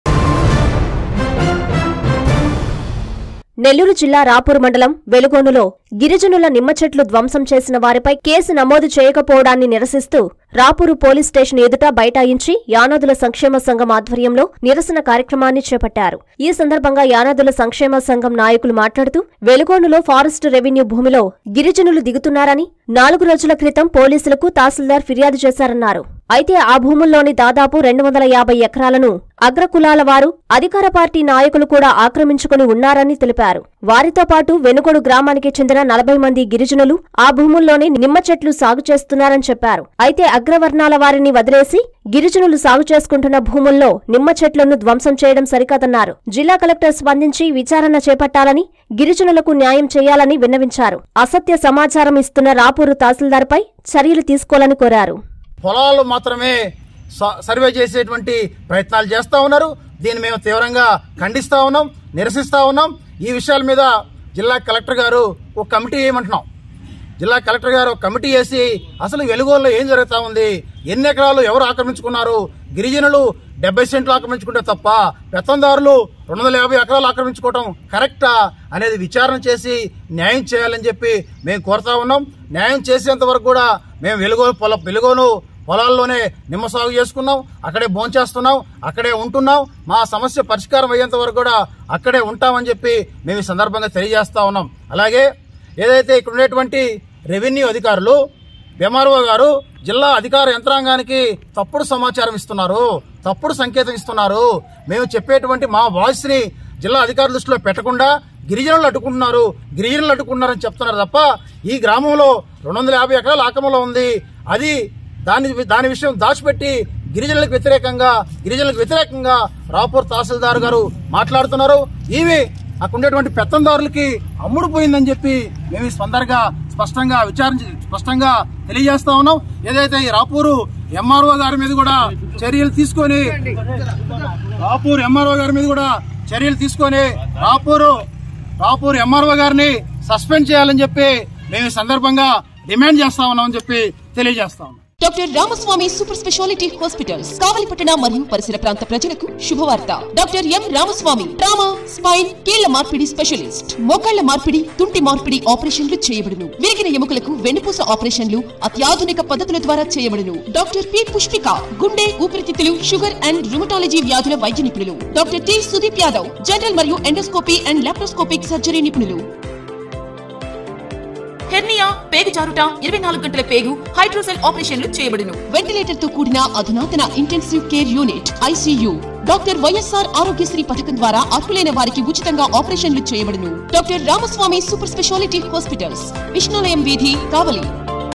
పోలీస్‌స్టేష‌న్ ఎదుట గిరిజ‌నుల నిర‌స‌న
వెలుగోనులో గిరిజ‌నుల నిమ్మ‌చెట్లు ధ్వంసం స‌రికాదు
నింధితుల‌పై ఎస్సీఎస్టీకేసులు న‌మోదు  చేయాలి
రాపూరులో ఆందోళ‌న ఉదృతం చేసిన యానాదుల సంక్షేమ సంఘం